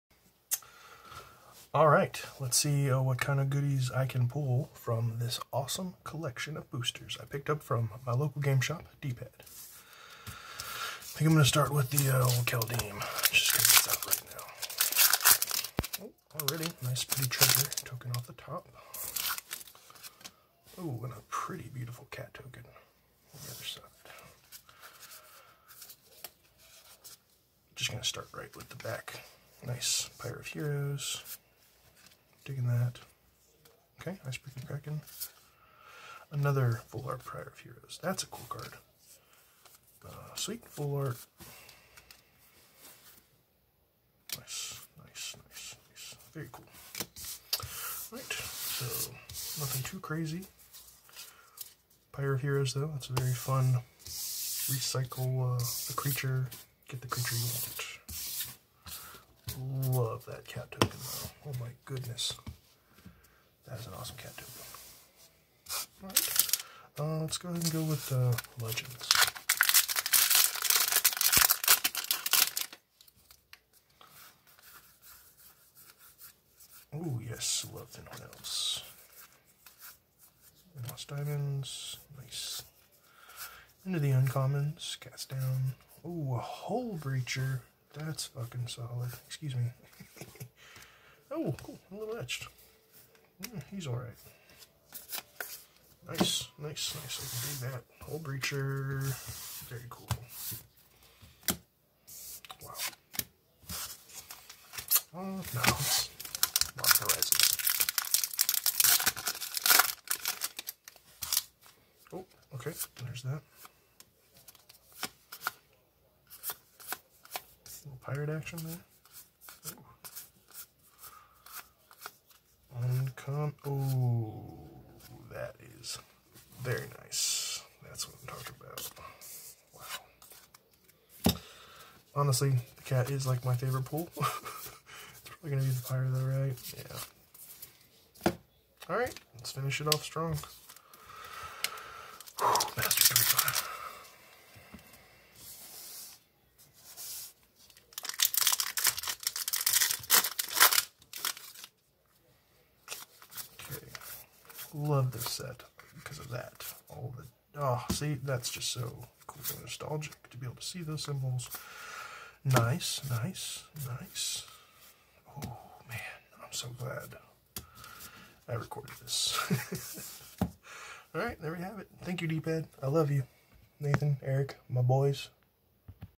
Magic: the gathering multi Pack opening
Thank you Dpad for the wonderful pulls as always. Here in New Braunfels TX!
Look em up and check out their Game room.

Love supporting my local game store.